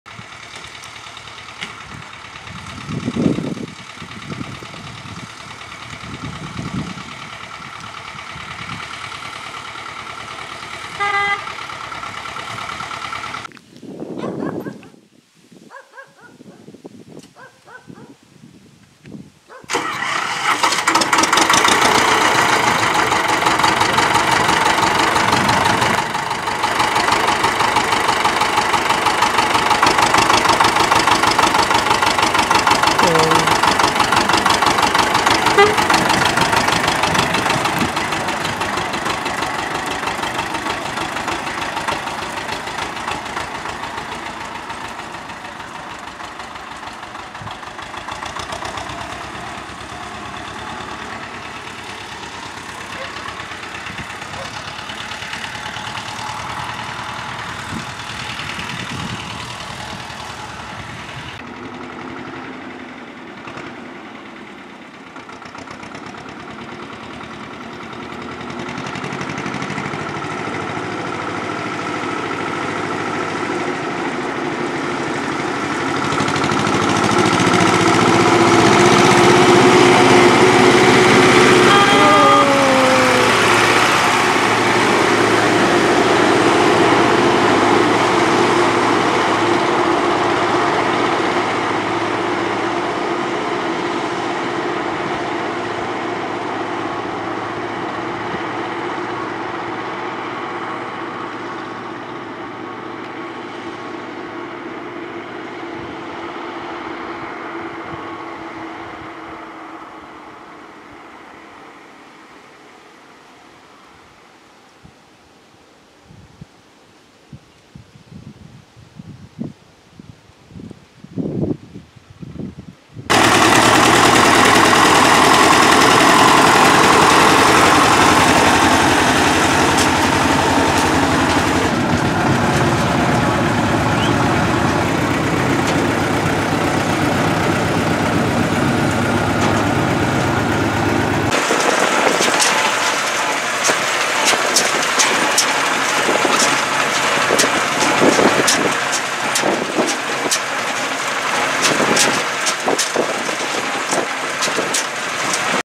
Друг купил трактор Т25
друг купил трактор Т25 владимеред втз с самодельныном прицепом одноосный и сцепка с трёх борон болшых. трактор покупался с проблемой двигателя давления на холостых 0 а на оборотов 0.8  под ремонт